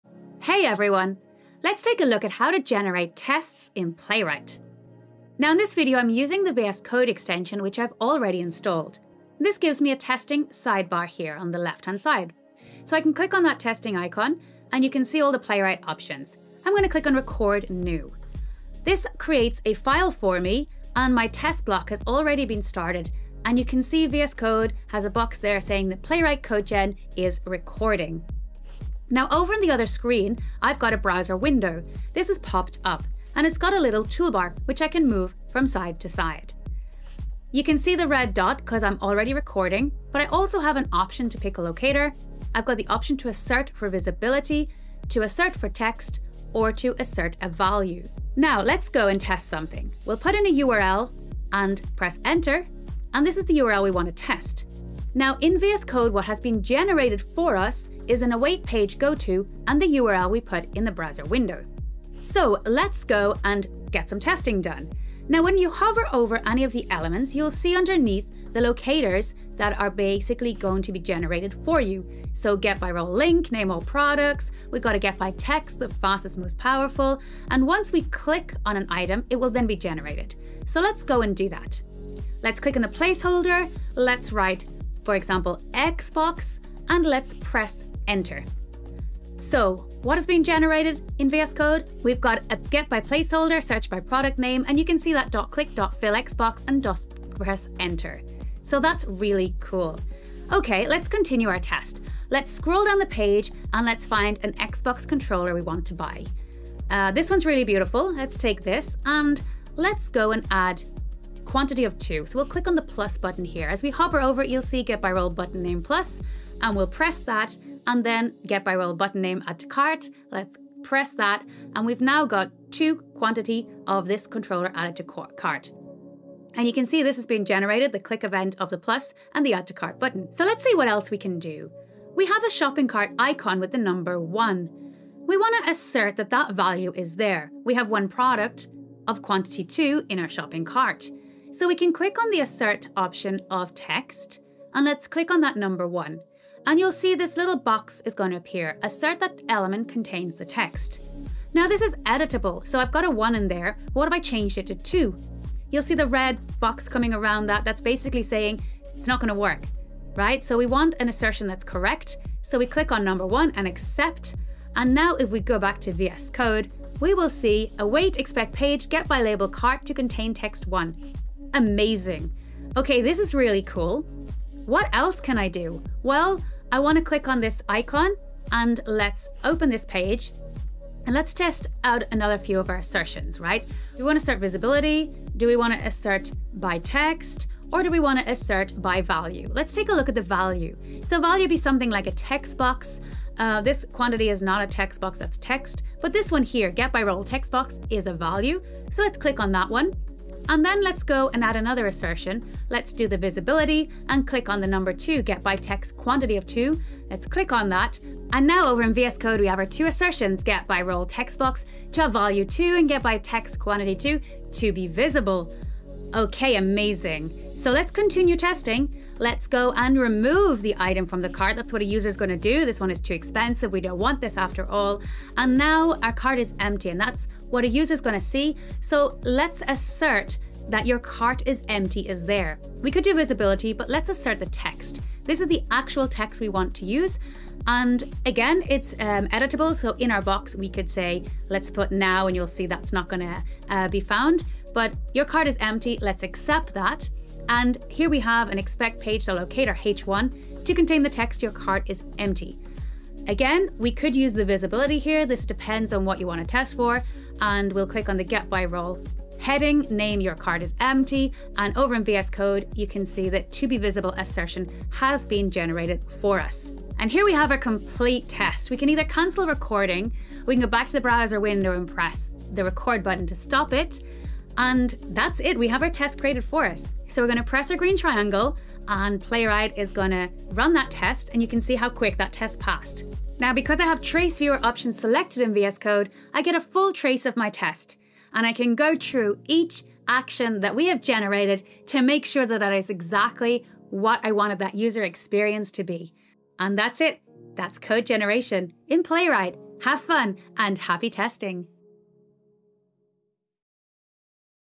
How to Generate Tests in Playwright with the VS Code Extension
Playwright will record your actions and generate the test code directly in VS Code. You can also generate assertions by choosing one of the icons in the toolbar and then clicking on an element on the page to assert against.